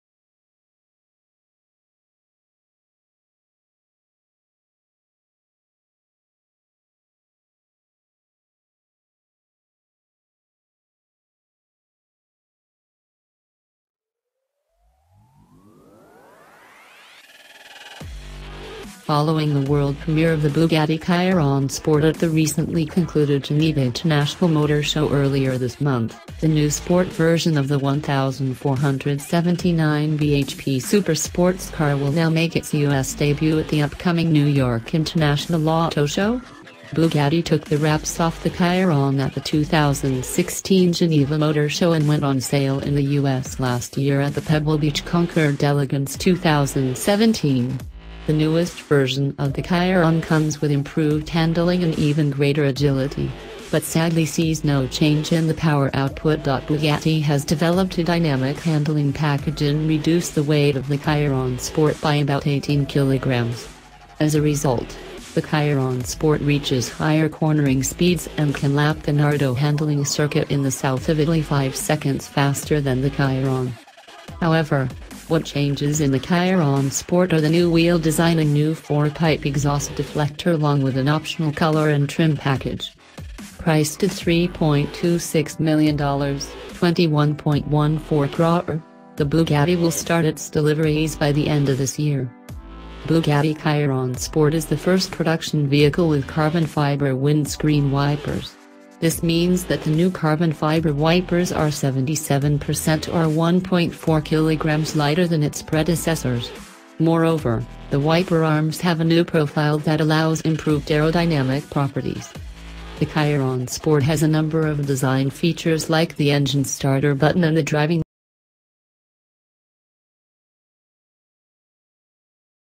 Hot News !!! Bugatti Chiron Sport Debut at 2018 Geneva Motor Show
Hot News !!! Bugatti Chiron Sport Debut at 2018 Geneva Motor Show -  Which means that, when Bugatti goes back to the Volkswagen Group’s Ehra-Lessien test track with the Chiron next year, to tell us exactly how fast it’ll go, it’ll be a bigger number than that official figure.

Bigger than the Veyron Super Sport’s number, Dürheimer says, by a notable amount, although nobody at Bugatti yet cares to speculate how fast that might be. If it were 10 percent faster - and with 50 percent more power, that’s not unreasonable - that’d be 295mph.

██████ GET DAILY UPDATES! NOW! ██████